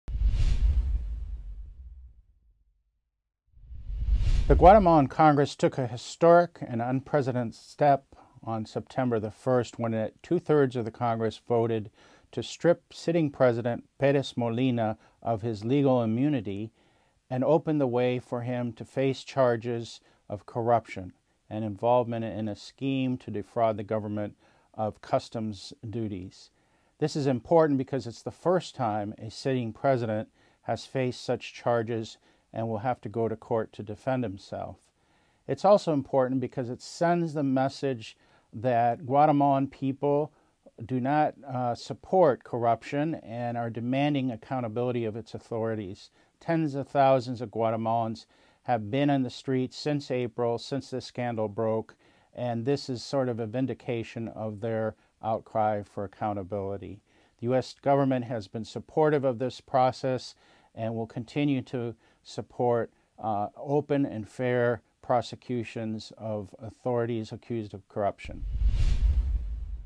Guatemalan President Stripped of Immunity
In an historic move, Guatemala’s legislature has stripped President Otto Perez Molina of immunity. Latin American Program Associate Director, Eric Olson, provides an overview of the situation.